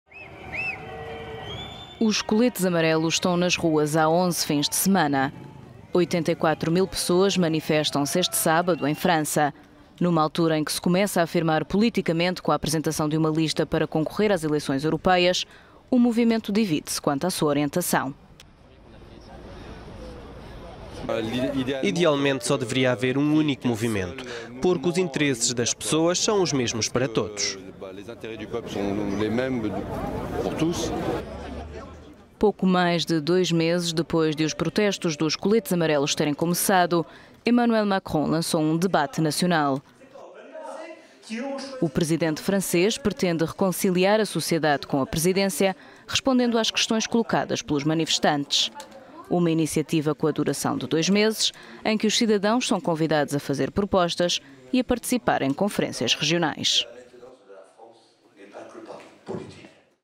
"Coletes Amrelos" voltam às ruas para Ato XI
84 mil "Coletes amarelos" protestam em França. Macron prossegue com debate nacional.…